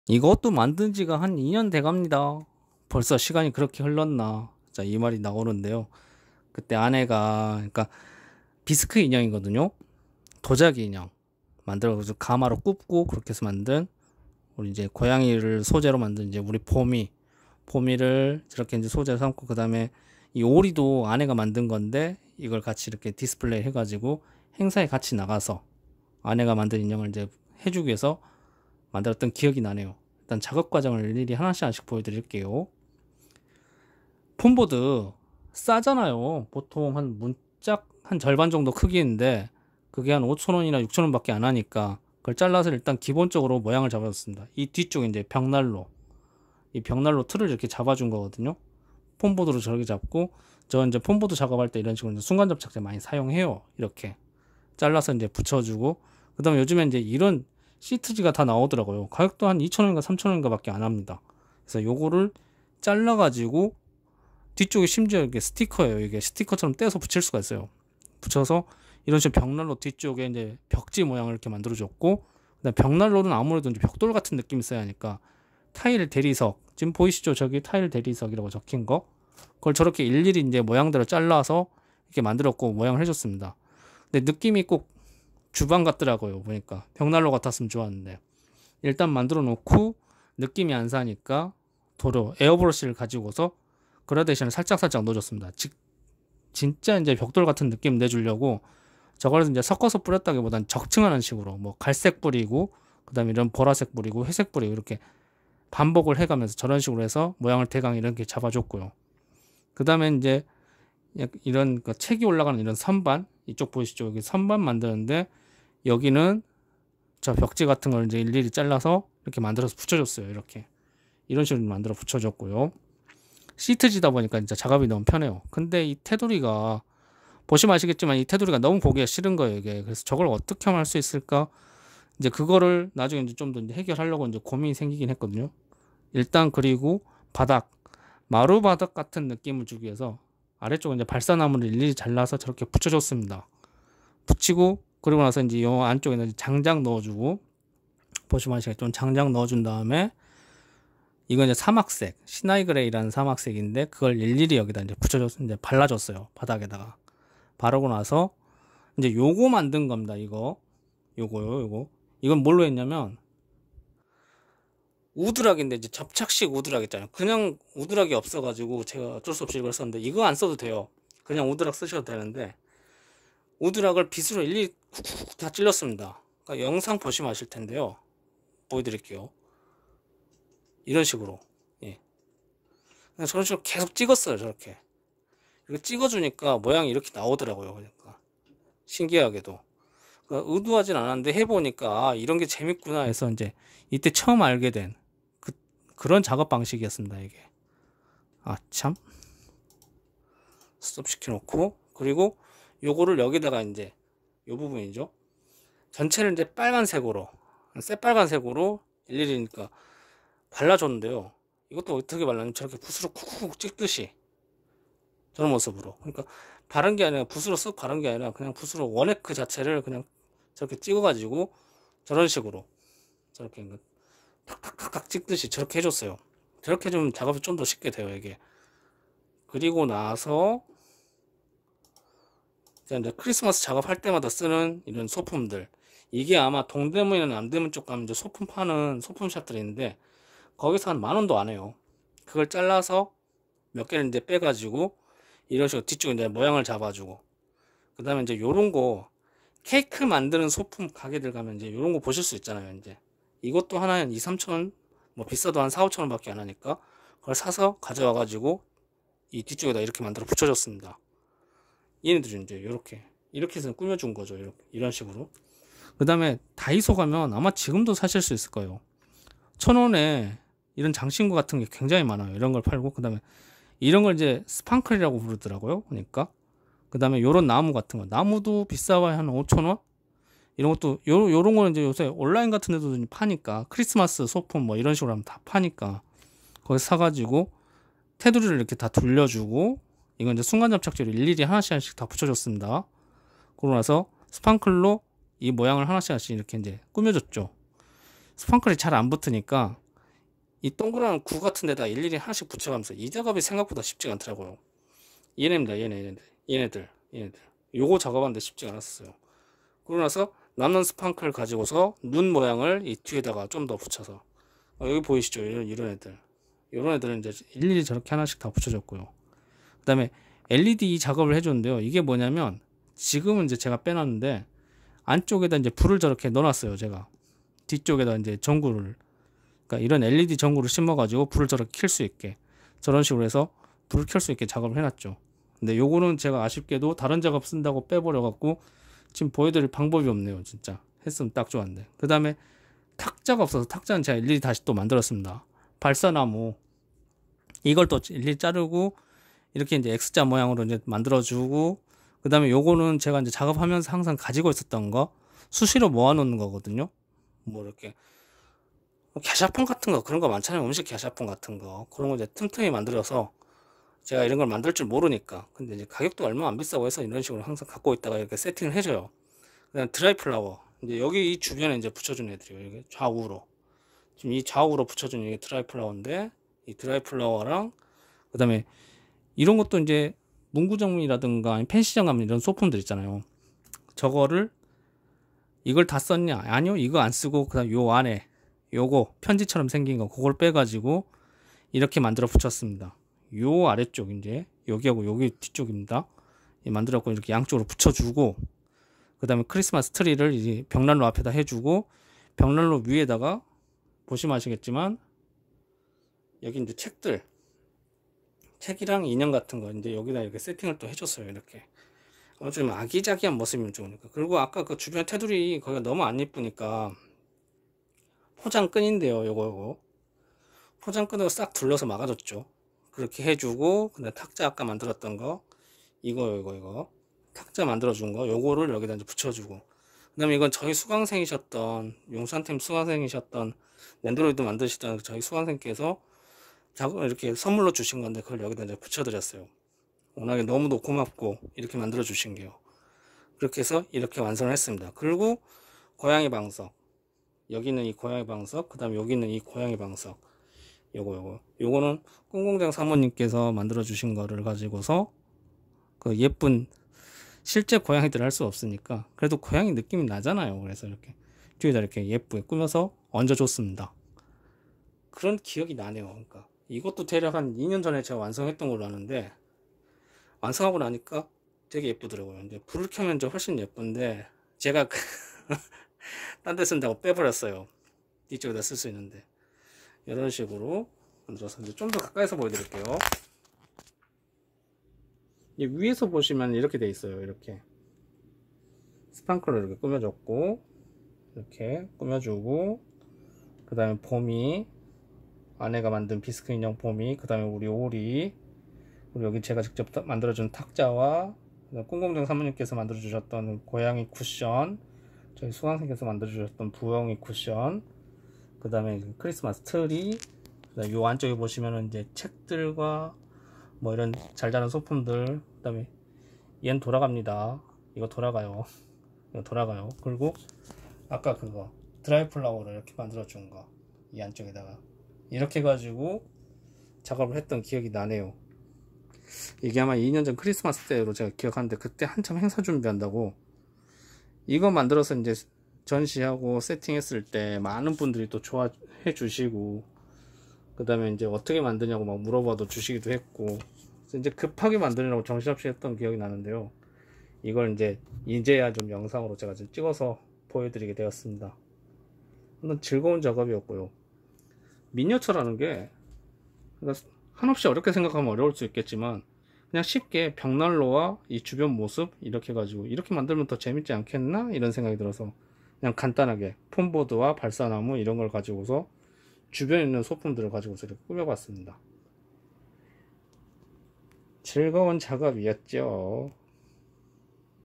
튜닝타임즈 프라모델 강좌 김두영 강사님의 Merry Christmas! 메리 크리스마스! the Fireplace of Bomi's Room 봄이네 방 벽난로
프라모델 에어브러시 수업과 넨도로이드 오비츠 커스텀 수업을 담당하고 있는 김두영 강사님이 2018년 크리스마스를 앞두고서 그해 12월에 마눌님의 비스크인형 고양이 봄이가 오리와 함께 벽난로 앞에 앉아서 홈파티를 하는 장면으로 미니어쳐 작업을 해주셨는데요, 프로젝트돌 행사를 앞두고서 수강생들과 함께 참가하려고 열씸히 만들어서 디스플레이를 해주셨던 미니어쳐 작품입니다^^ 튜닝타임즈